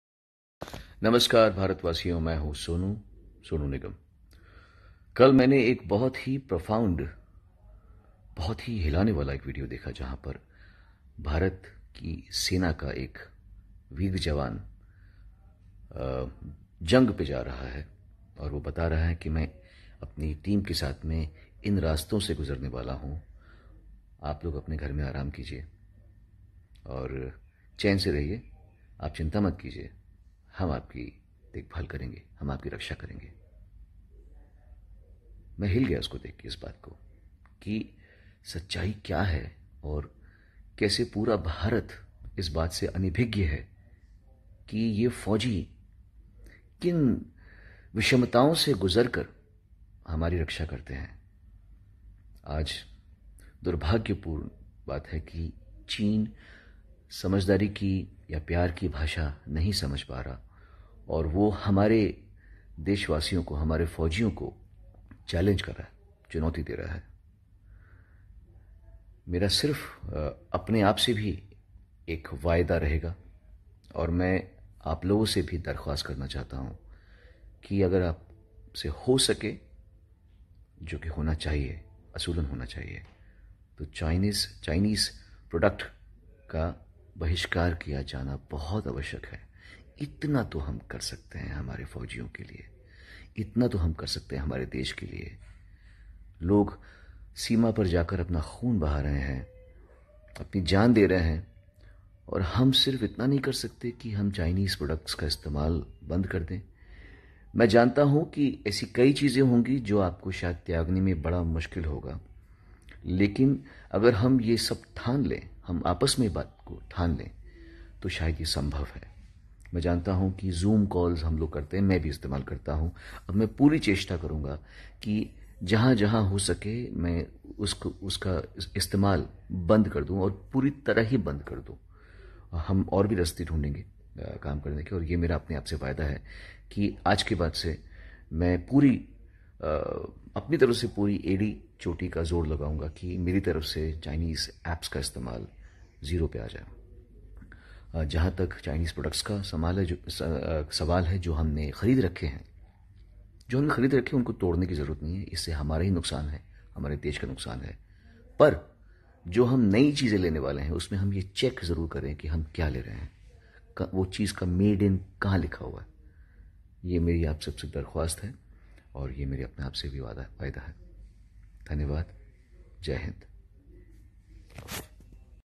Now Sonu nigam urges people to boycott china products.
Recently Sonu nigam urges people to boycott china products.